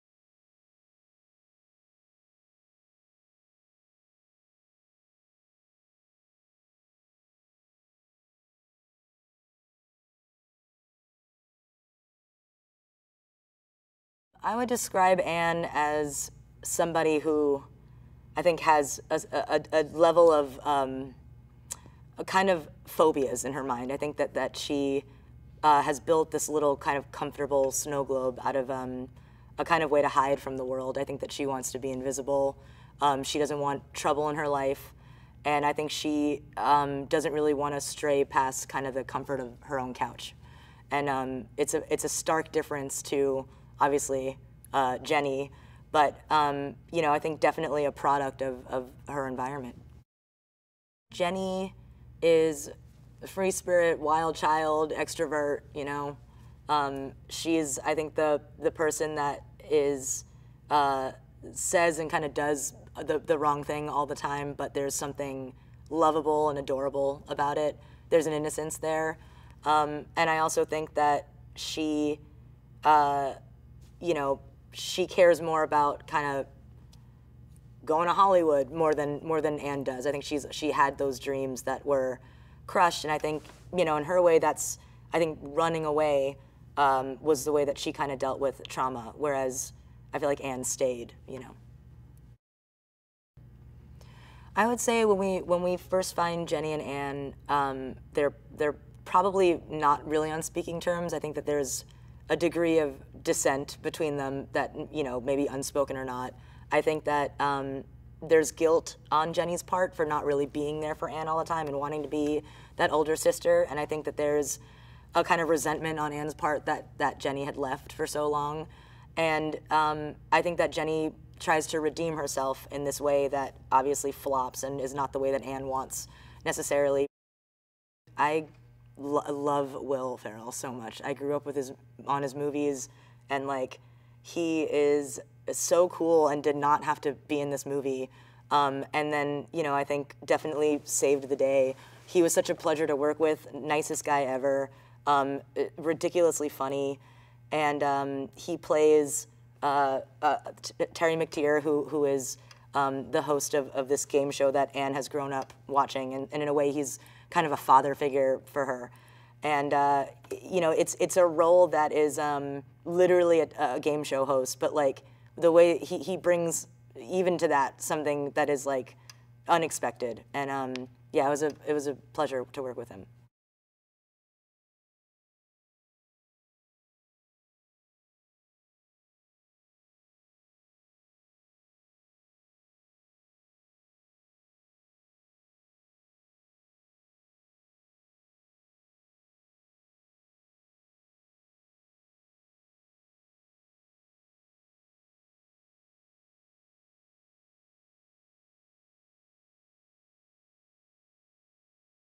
"Quiz Lady" Interview with Awkwafina - Producer /"Anne"
(20th Century Studios)(Hulu)
Release 11/3/23
     A gameshow-obsessed woman and her estranged sister work together to help cover their mother's gambling debts.
     Cast: Awkwafina, Will Ferrell, Holland Taylor, Jason Schwartzman, Sandra Oh, Tony Hale,
Angela Trimbur, Ginger Cressman
     Director: Jessica Yu